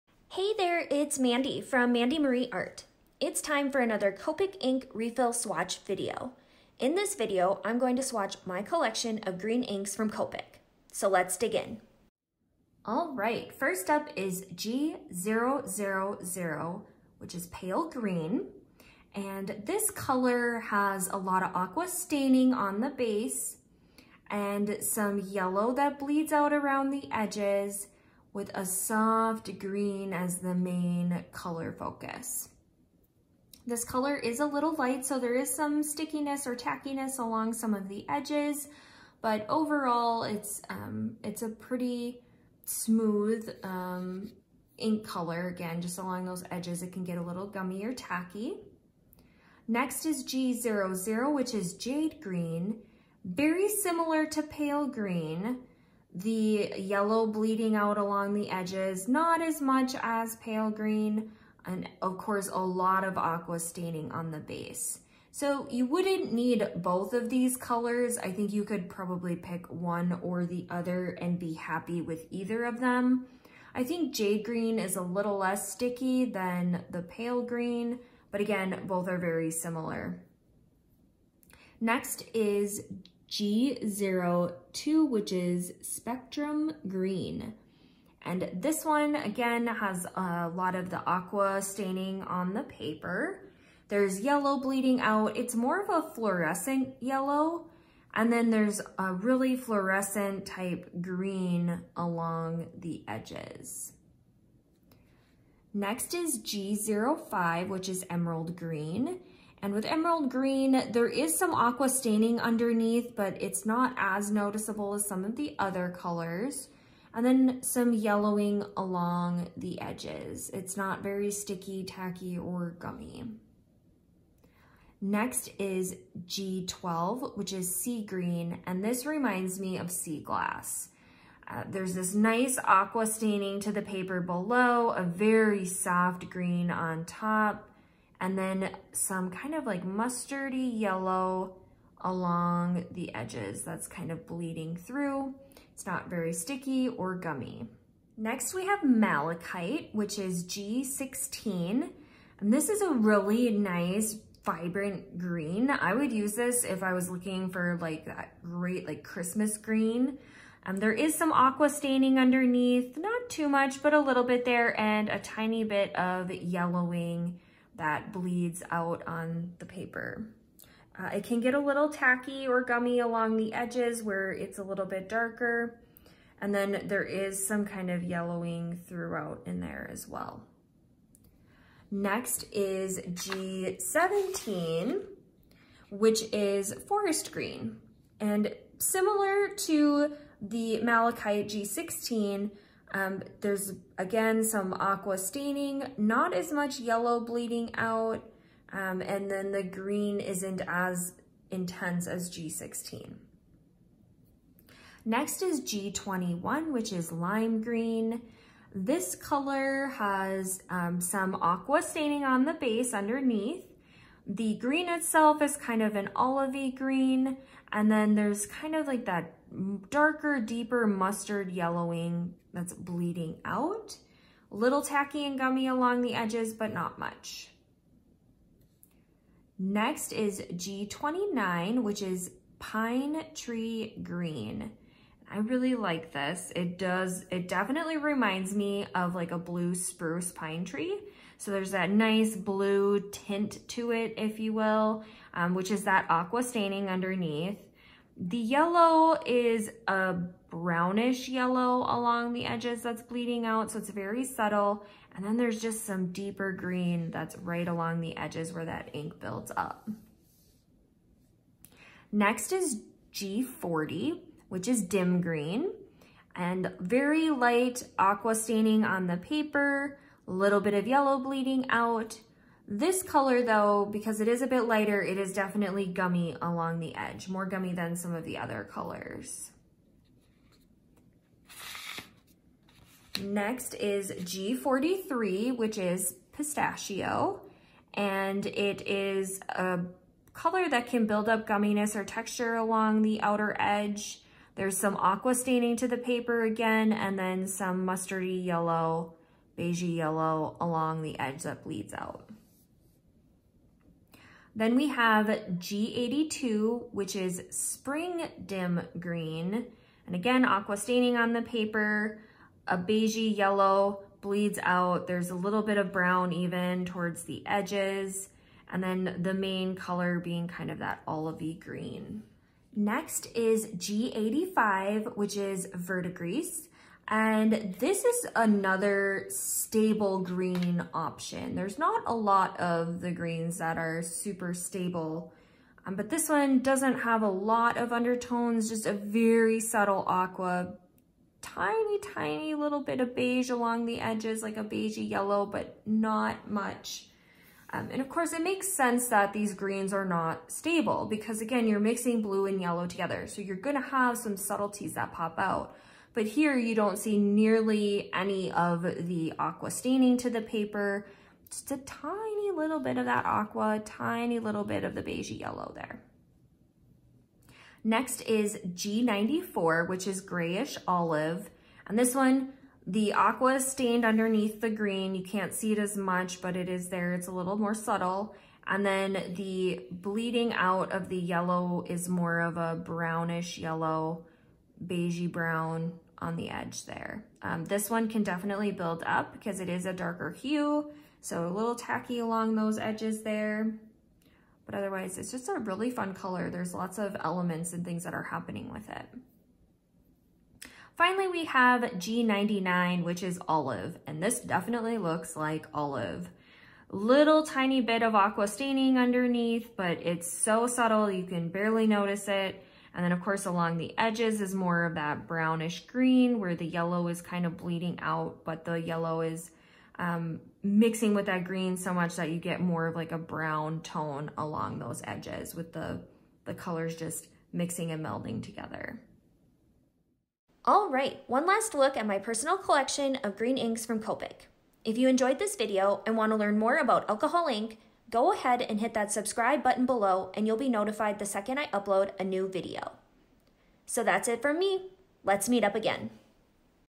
15 Envious Green Copic Alcohol Ink Refill Swatches
Thank you for stopping by! In this video, I swatch 15 Copic refill inks from the Green Series from Copic Ink.

You'll find a detailed tutorial including swatches of Copic refill inks from the Green series and information about the texture and undertones for each color.

Each color is swatched on medium weight yupo paper from Legion Brand using 91% isopropyl alcohol to dilute and move the ink on the page.

Colors used:
G0000 Pale Green
G00 Jade Green
G02 Spectrum Green
G05 Emerald Green
G12 Sea Green
G16 Malachite
G17 Forest Green
G21 Lime Green
G29 Pine Tree Green
G40 Dim Green
G43 Pistachio
G82 Spring Dim Green
G85 Verdigris
G94 Grayish Olive
G99 Olive

Want to learn how to decipher the numbering and lettering system for Copic Ink Refills?